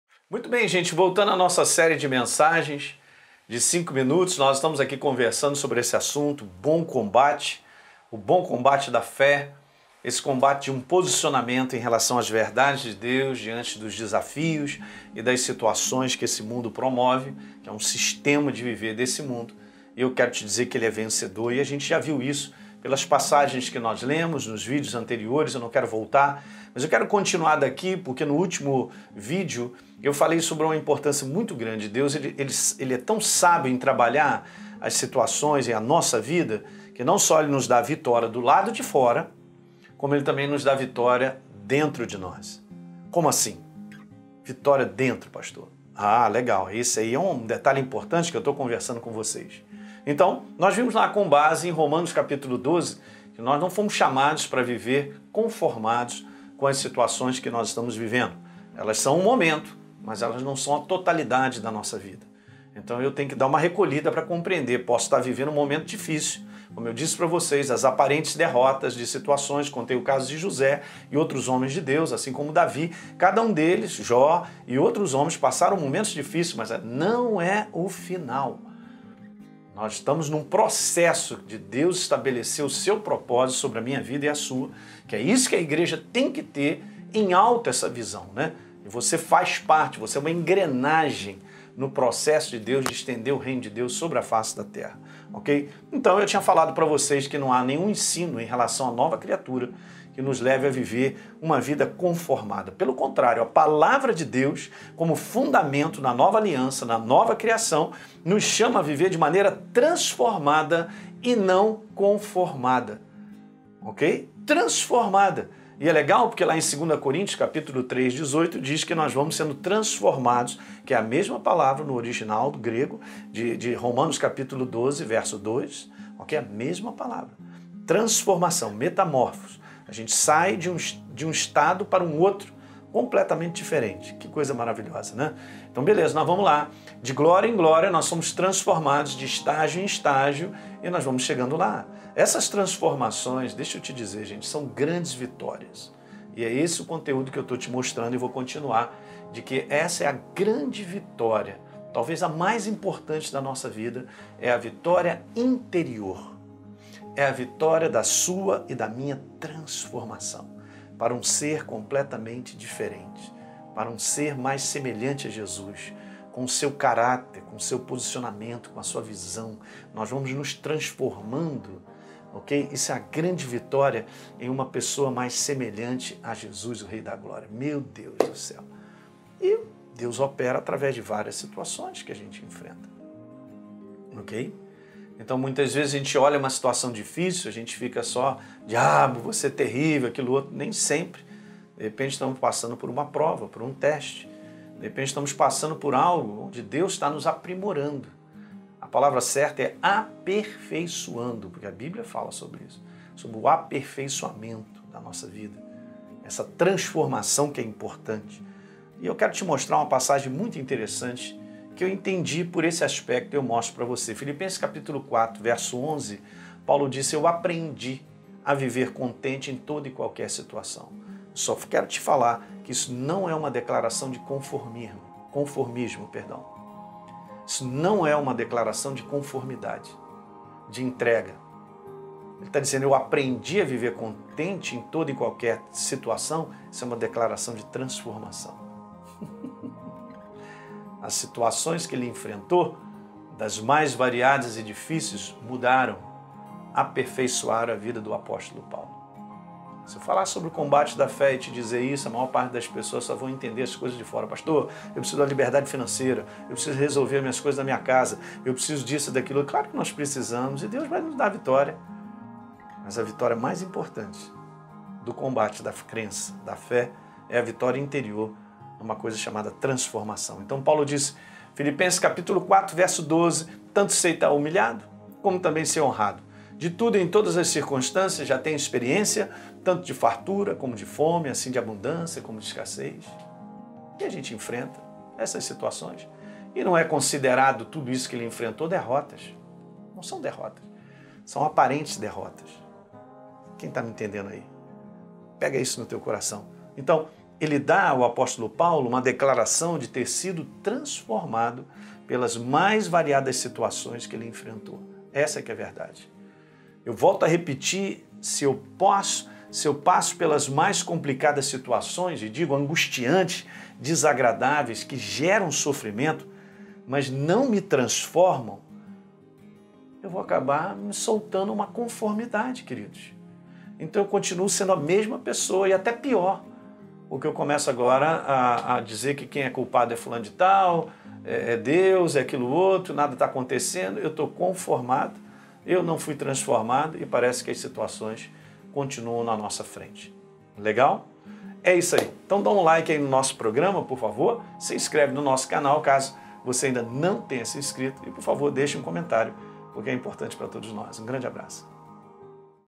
Vitoriosos por dentro e por fora | Helio Peixoto | #6
Quando falamos em combate da fé, a maioria das pessoas pensa em resolver problemas financeiros, familiares, de saúde. É claro que precisamos de soluções, e Deus vai nos dar a vitória nessas áreas, mas o mais importante no combate da fé é a vitória interior, fruto de uma vida aperfeiçoada e mais semelhante a Jesus.

➤ Spotify: Exerça sua Fé com Helio Peixoto